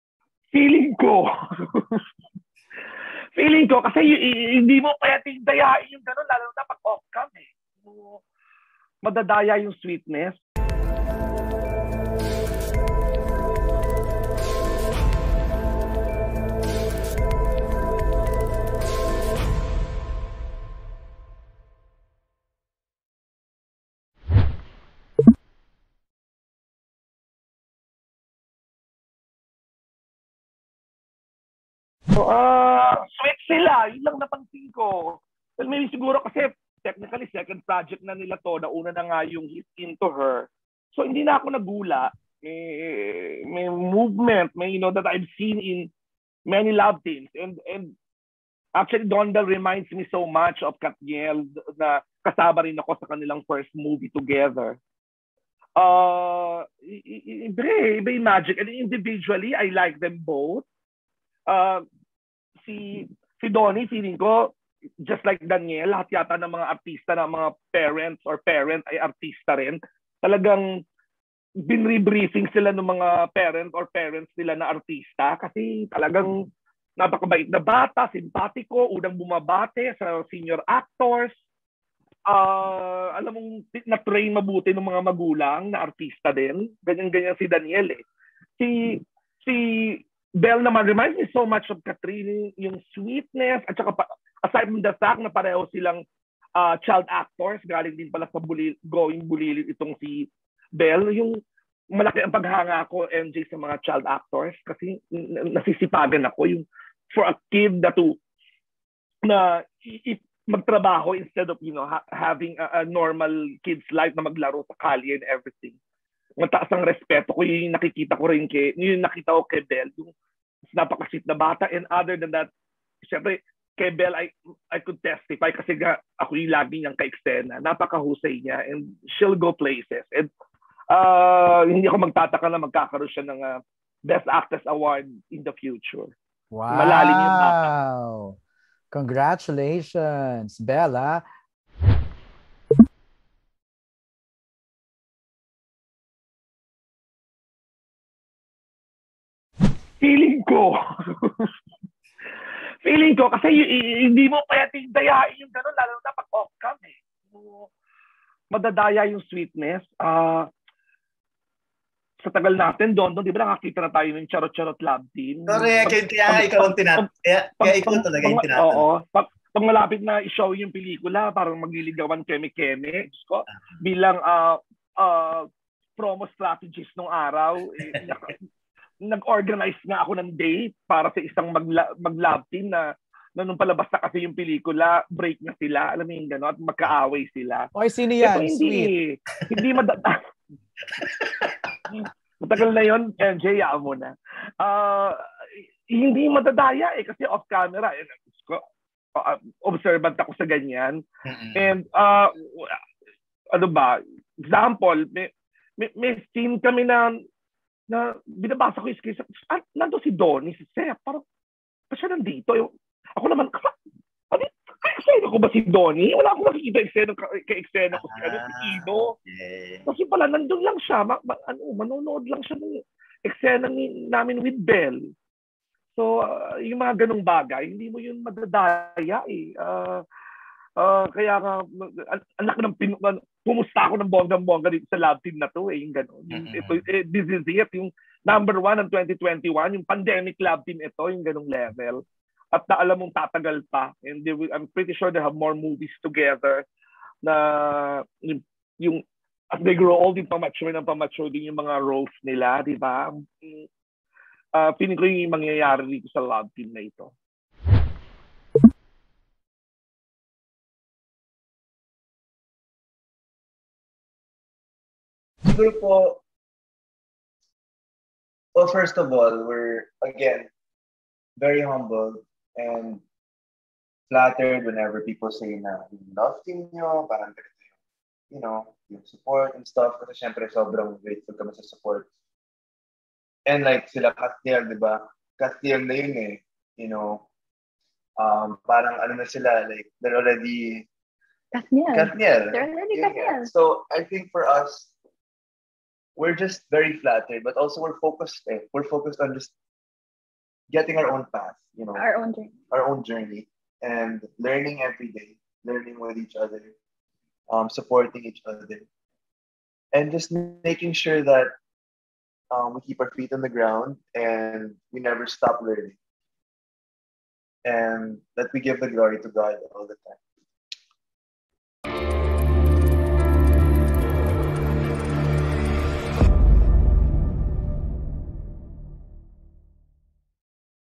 John Lapus hindi na nagulat sa sweetness ng DonBelle, sees KathNiel in them
DonBelle's sweetness both on-cam and off-cam made actor-director John "Sweet" Lapus on Thursday, December 2 believe that the tandem could be the next KathNiel.